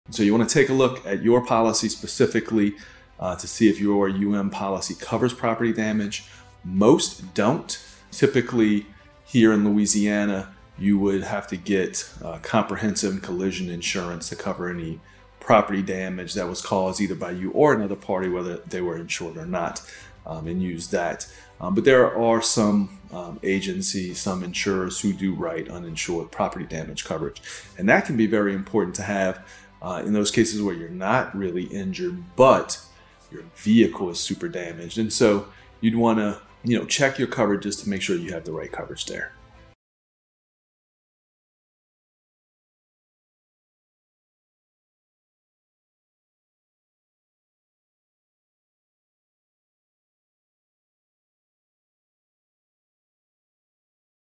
Maximize Your Car Insurance: Understanding UM Policy and Collision Coverage in Louisiana
Discover the essentials of Uninsured Motorist (UM) Policy and comprehensive collision coverage in Louisiana. This YouTube Short guides you through what your policy covers, the importance of property damage protection, and how to ensure your vehicle is fully protected. Whether you're in an accident or facing vehicle damage, know what your insurance covers.